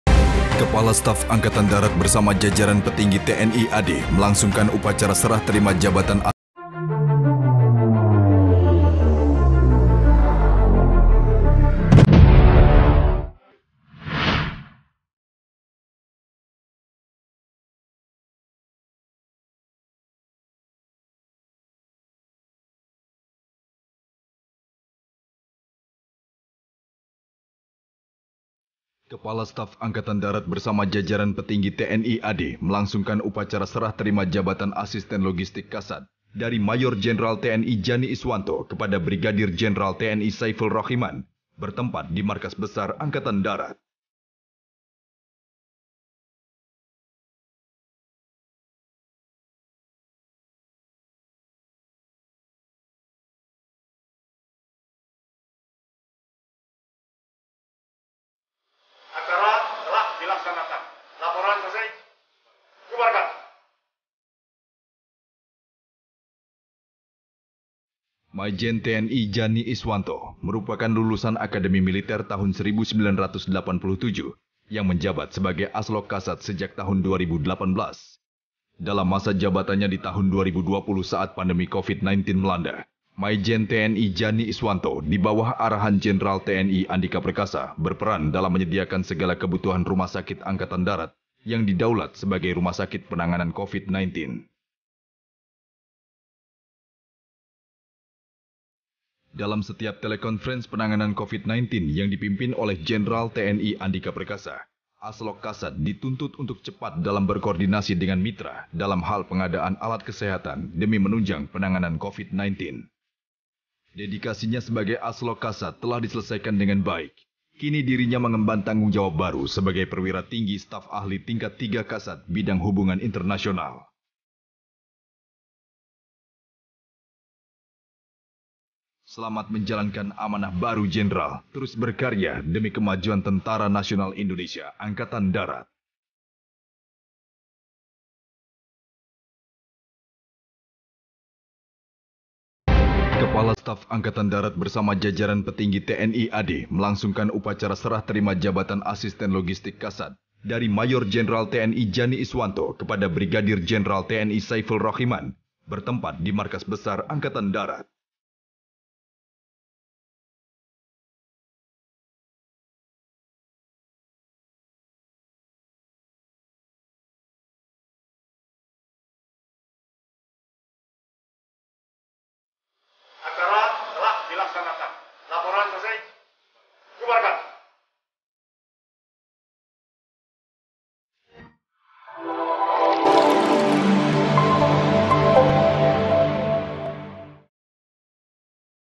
Sebelum Resmi Jadi Panglima Baru, Andika Perkasa Lantik Jenderal Terakhir Ini di Jajaran TNI AD
TRIBUN-MEDAN.com, MEDAN - Menjelang detik-detik KSAD Jenderal TNI Andika Perkasa akan dilantik presiden menjadi Panglima TNI baru, ia melantik Brigjen TNI Saiful Rachiman menjadi Asisten Logistik (Aslog) Kasad.

Pelantikan ini akan menjadi yang terakhir bagi Andika melakukan pelantikan di jajaran TNI Angkatan Darat, sebelum akhirnya akan dilantik Presiden RI menjadi panglima hari ini, Rabu (17/11/2021).

Lewat postingan akun youtube resmi TNI AD, Rabu (17/11/2021) Jenderal Andika memimpin upacara Serah Terima Jabatan Asisten Logistik (Aslog) Kasad, dari Mayjen TNI Jani Iswanto, kepada Brigjen TNI Saiful Rachiman, di Markas Besar Angkatan Darat (Mabesad).

Selanjutnya Mayjen TNI Jani Iswanto akan menempati jabatan sebagai Pati Sahli Tingkat III Kasad Bidang Hubungan Internasional.

Sedangkan Brigjen TNI Syaiful Rachiman sebelumnya menjabat Waaslog Kasad Bidang Perencanaan Pembinaan Administrasi Logistik.